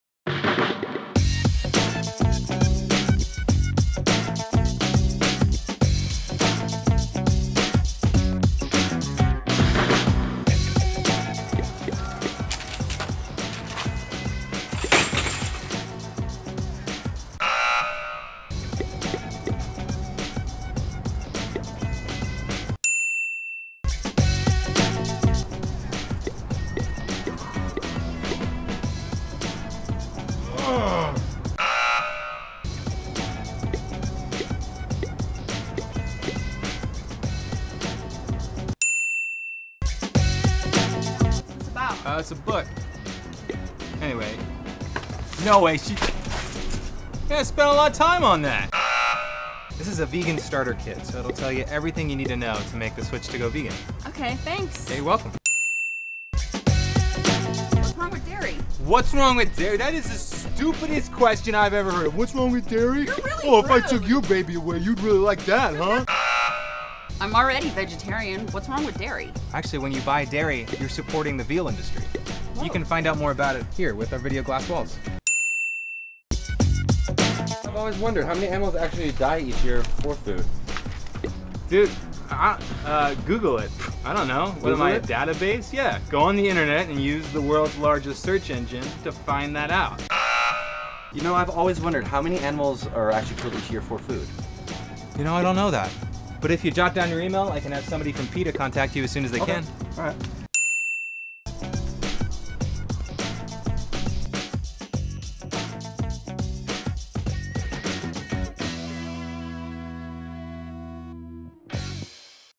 Tabling in Five Easy Steps
Tabling is an extremely effective way to get active for animals. Here's how you do it.

Join PETA's Action Team for more tips on how you can help animals